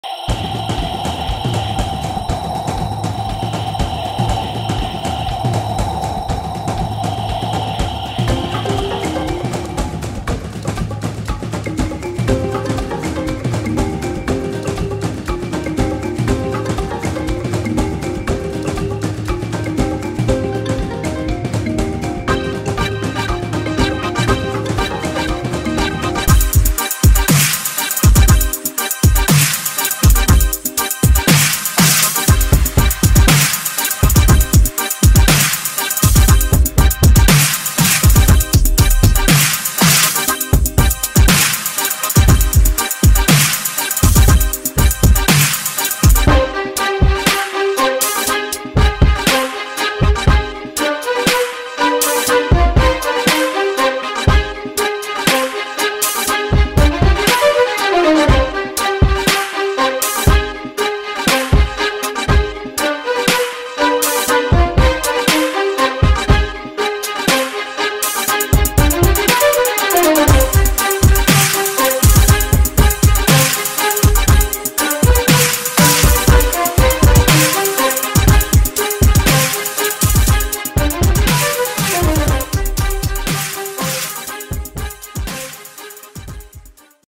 My parkour in 2020 AGGATEN بعض اللقطات في الفديوهات parkour AGGATEN
بعض القطاتي من الفديوهات AGGATEN
ما رئيكم في الموسيقة انها من انتاجي